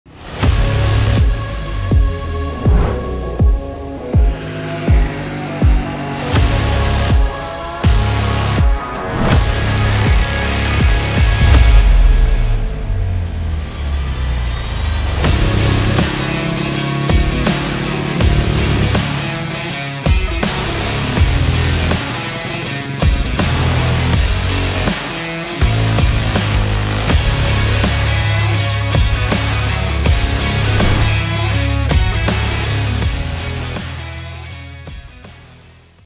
ROTTWEILER PERFORMANCE - OFFICIAL ATHENA GET ECU DISTRIBUTOR
Now distributing @getdatait Athena GET ECU's! Check it all out at ROTTWEILERPERFORMANCE.COM 👏 The GET ECU is a plug and play replacement ECU System (for Honda, Kawasaki, KTM, and Husqvarna four-strokes) that can be fully customizable to meet the demands of a stock or modified bike and is the best way to improve power and response.